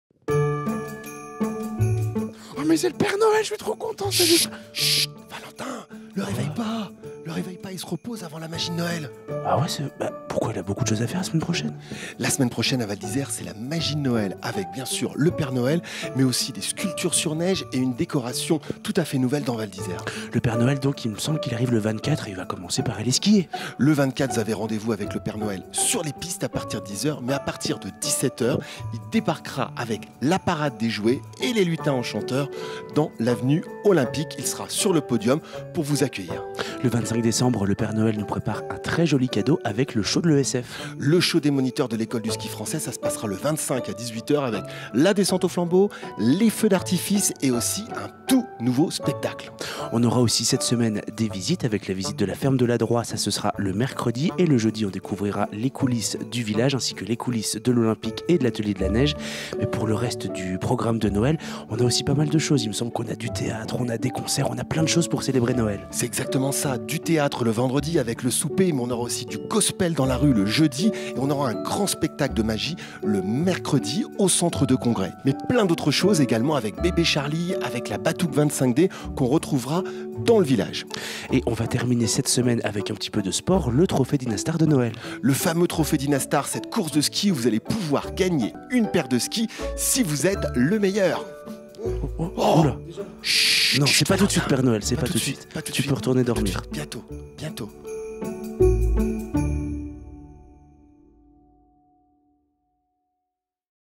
Les animations de la semaine de Noël à Val d'Isère c'est magique!
Découvrez le programme animations de votre semaine de Noël et retrouvez le Père Noël dans les rues et sur les pistes de Val d'Isère !
Venez admirer les différents spectacles, les sculptures sur neige et les décorations de Noël !